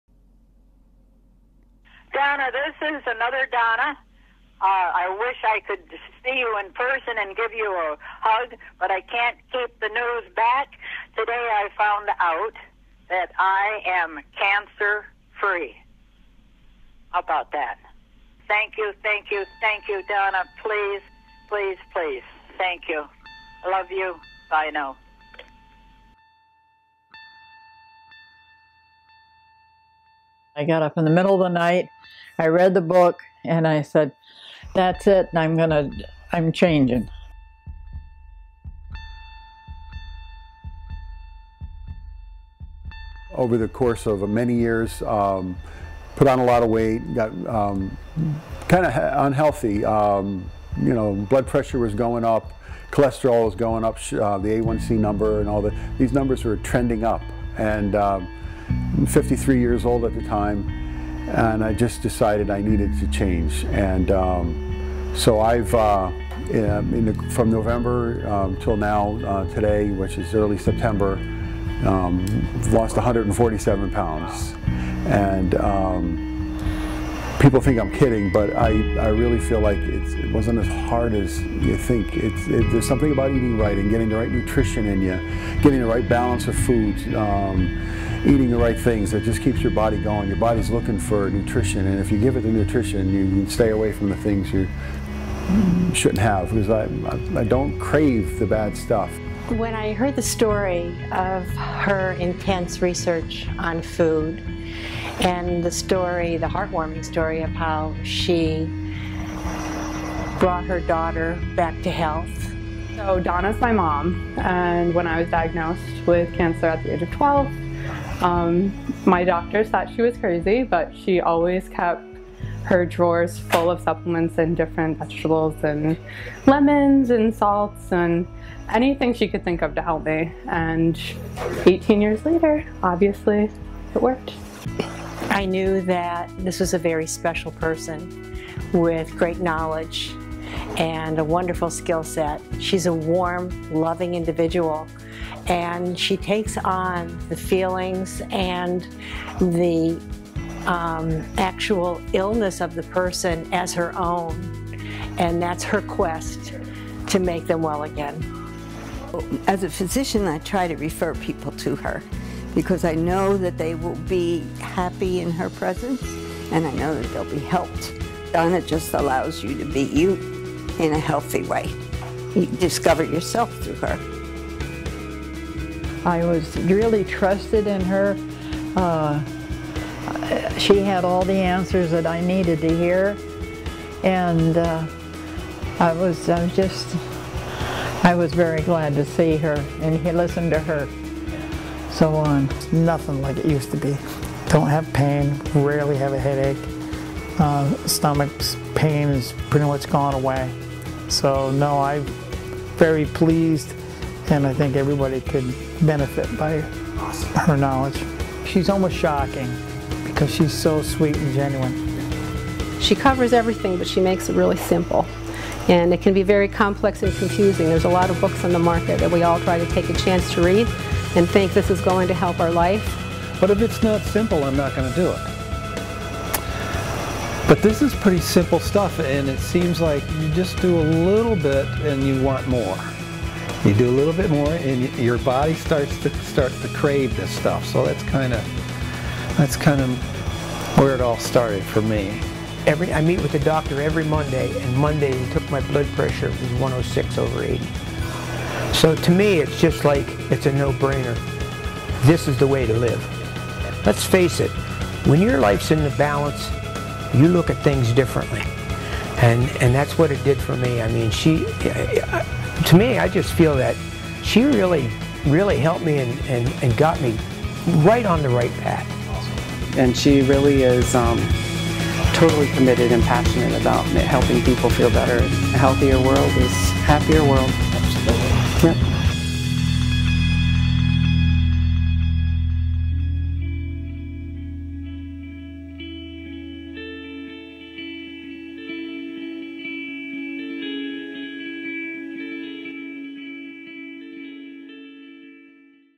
Simple. Natural. Healing. Book Trailer
Discouraged about your health?  LaBa explains approaches and new ideas so you learn quickly how to boost your body’s ability to heal itself.

It is totally amazing health transformation information on healing faster, improving lab reports, losing weight naturally, eat for healing. Anti-inflammatory eating, learning what hurts progress and what helps.   Donna makes transforming your health easy!

Never before explained this way! This book is the being of a new way to be healthy naturally.